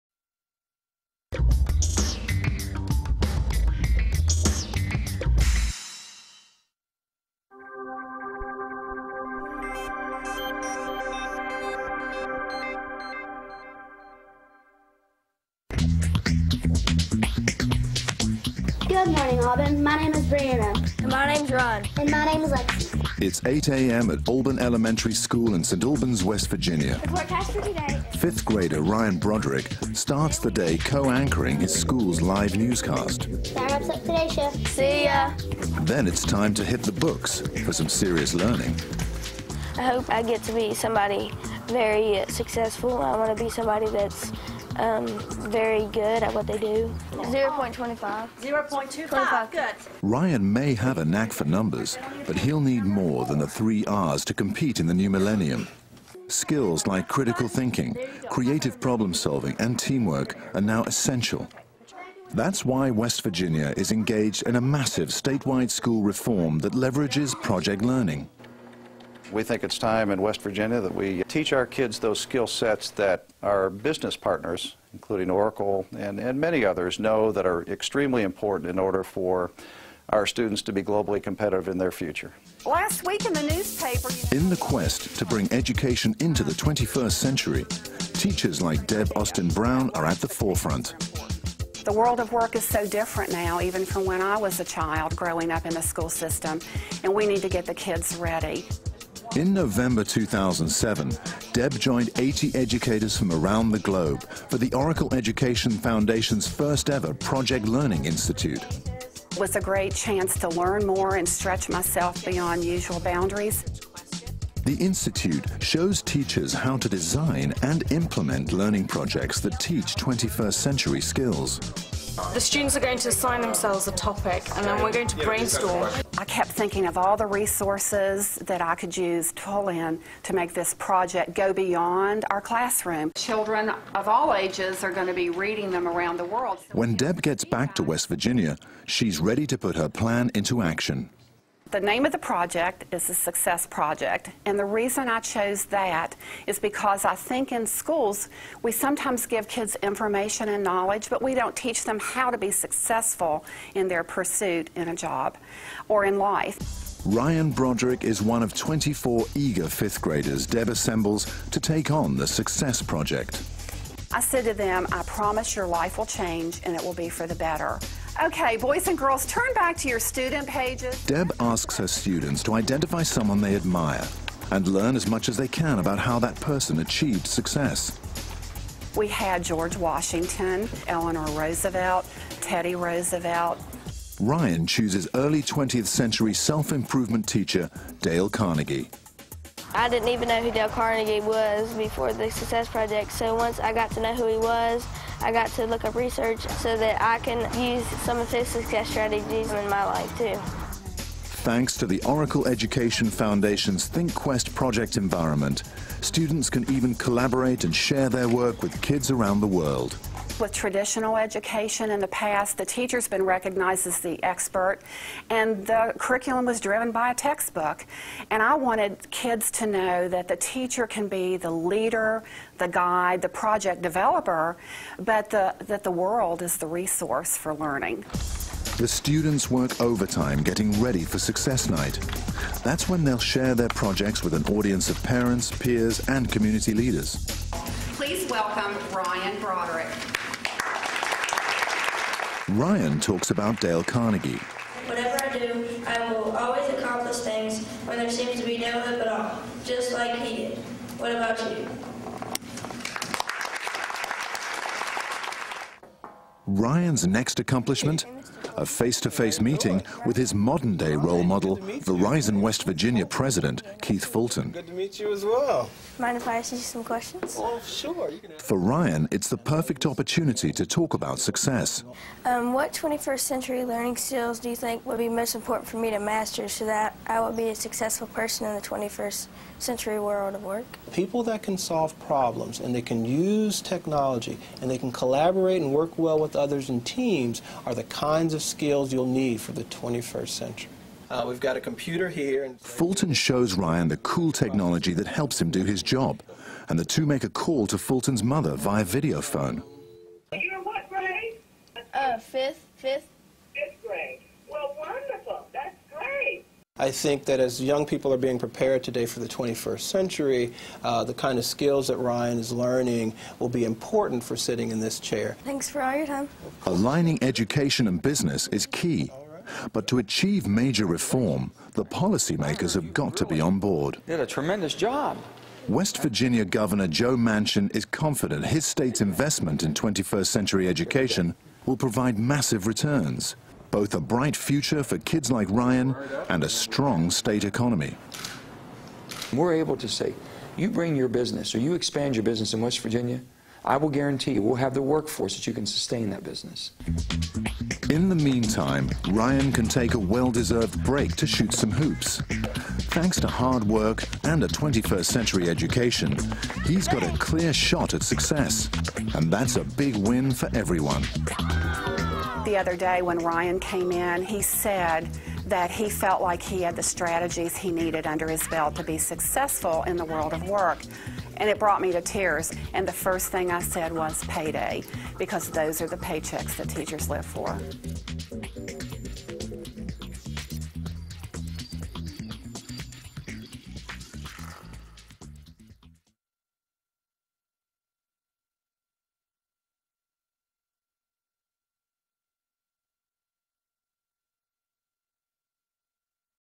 Oracle ThinkQuest Institute
Fifth graders from Alban Elementary School in West Virginia use ThinkQuest to complete their Success Project which goes beyond the classroom.